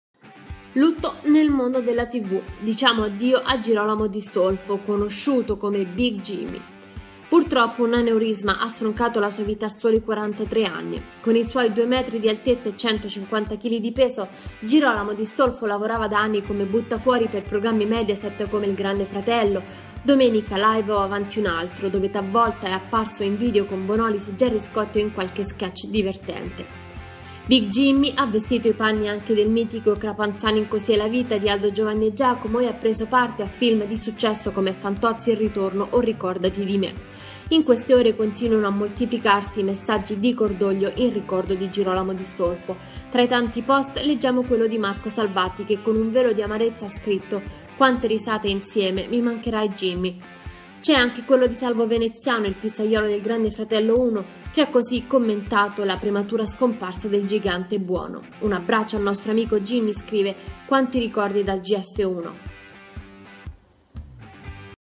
Lutto nel mondo della tv, addio al gigante buono Big Jimmy
Girolamo Di Stolfo – conosciuto come Big Jimmy – stroncato da un aneurisma